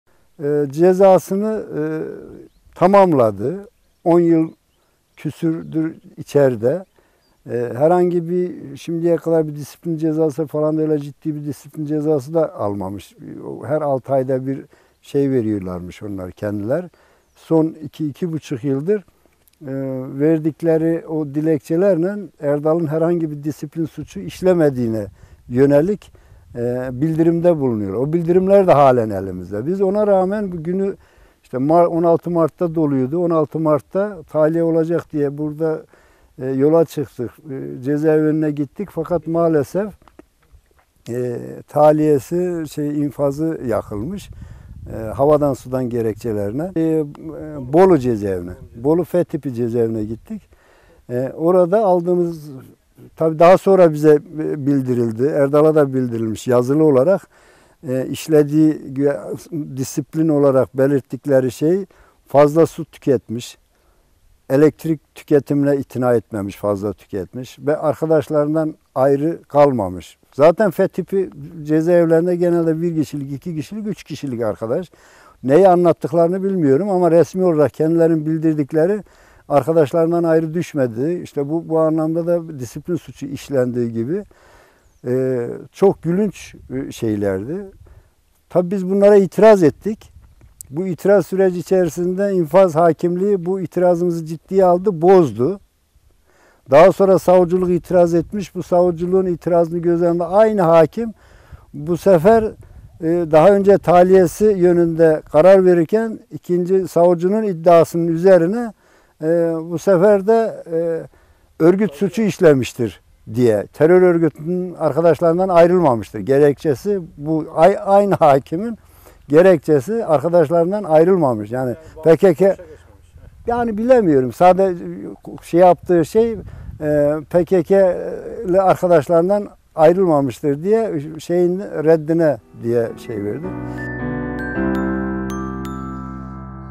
İNFAZI YAKILDI: GEREKÇESİ ARKADAŞLARINDAN KOPMAMA
DBP eski Eş Genel Başkanı Sebahat Tuncel'in kardeşi Erdal Tuncel'e verilen 13 yıl 6 ay hapis cezası bitmesine rağmen infazı "Arkadaşlarından kopmadığı" gerekçesiyle yakılarak, tahliyesi engellendi